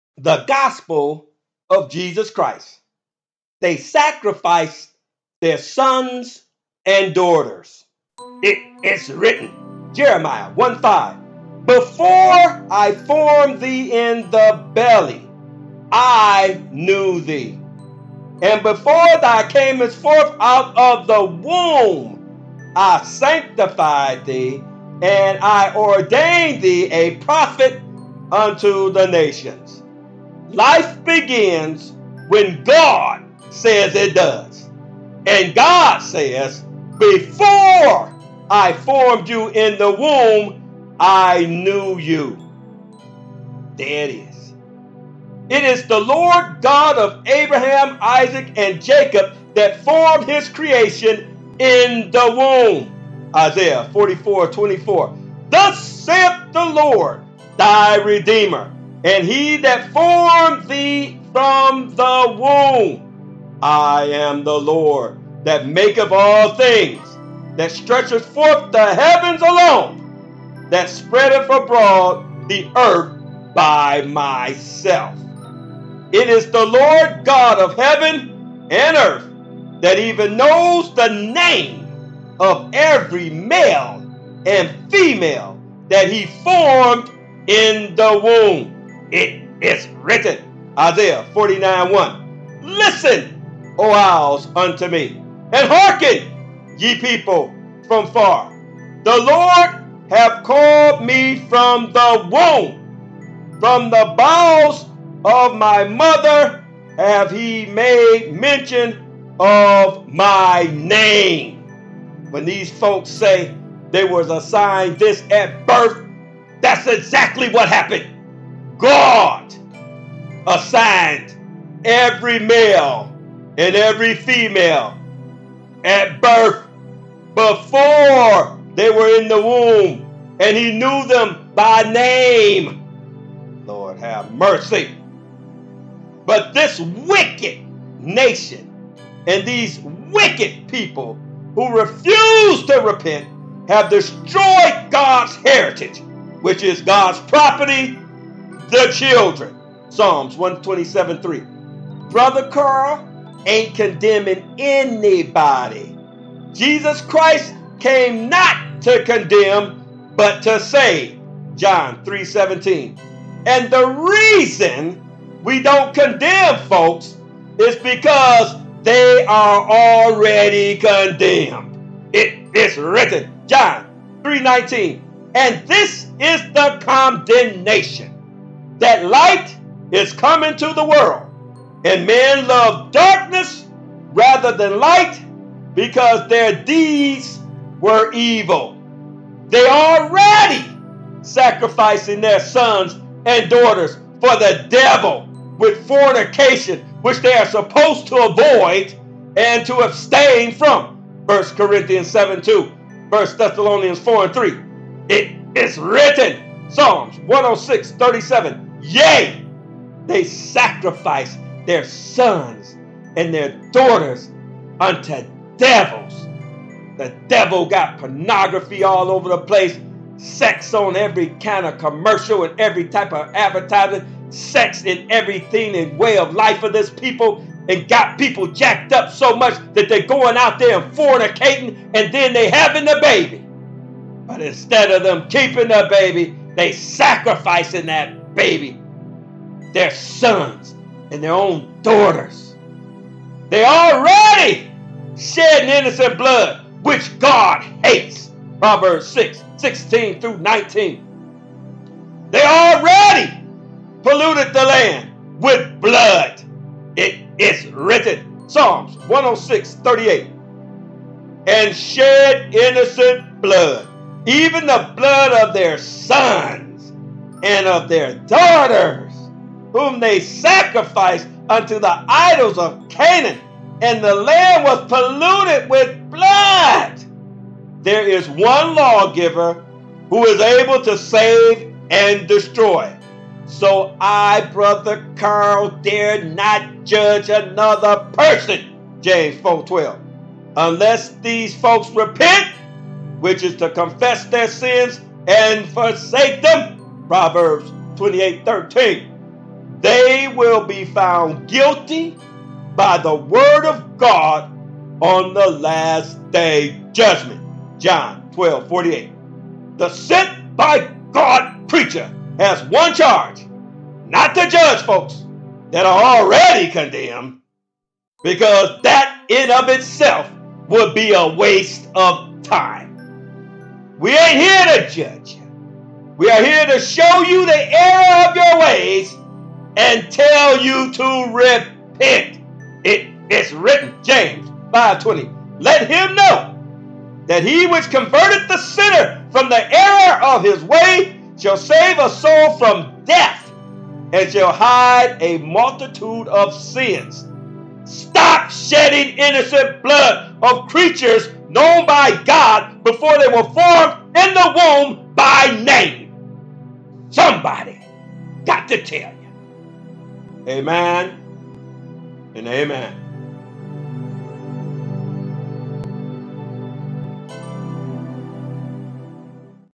The Gospel of Jesus Christ..."They Sacrificed Their Sons And Daughters"
The Gospel of Jesus Christ pleading with mankind to stop this error of shedding innocent blood. Telling them to flee and abstain from fornication for the land is polluted with blood. The question is will they repent in time for Judgment Day?